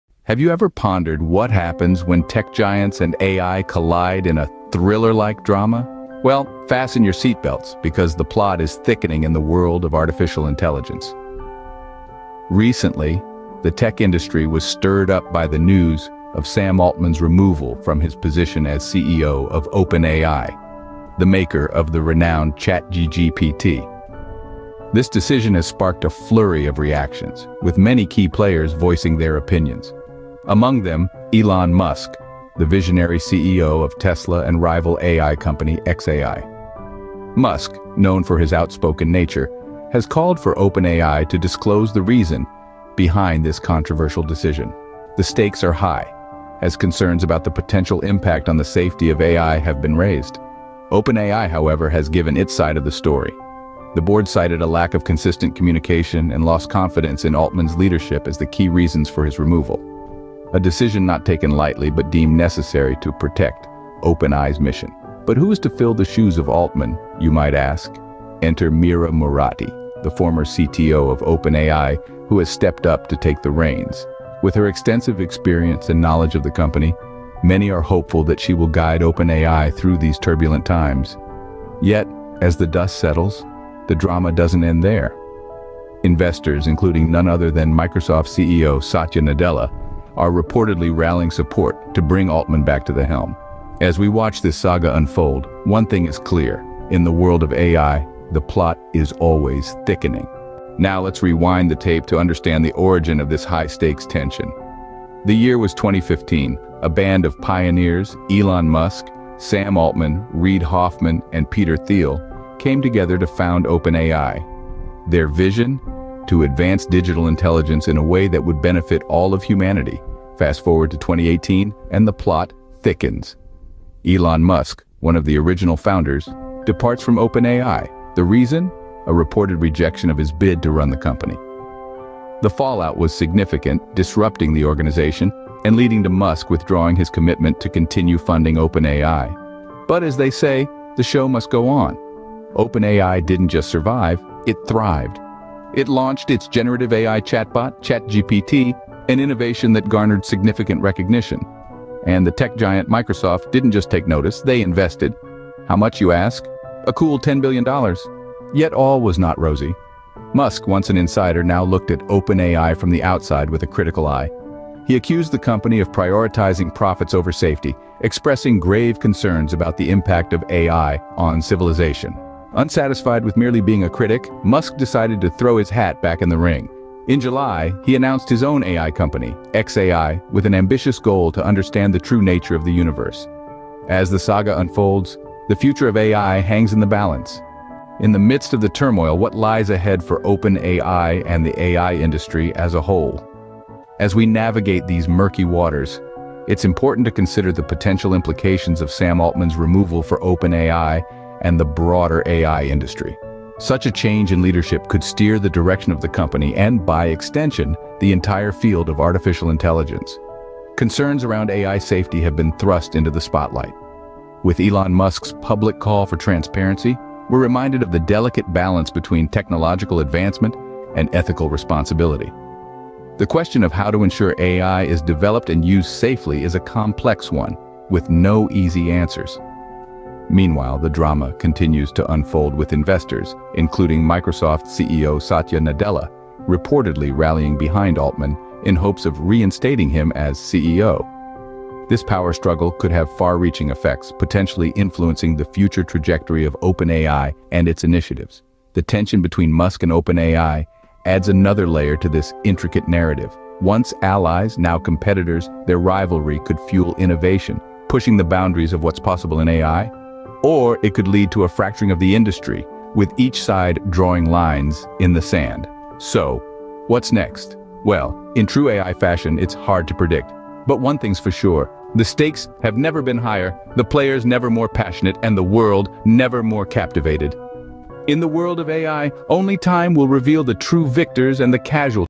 AI Drama Unfolds: Musk, Altman, and the OpenAI Controversy
In this thought-provoking video, we delve deep into the recent controversy that has been making waves in the tech community: OpenAI's surprising move to oust Sam Altman as CEO and Elon Musk's demand for transparency. Join us as we dissect the details, analyze the implications, and discuss the potential impact on the future of AI.

OUTLINE:

00:00:00 The Plot Thickens in AI World
00:01:40 The History and the Fallout
00:03:07 The Next Chapter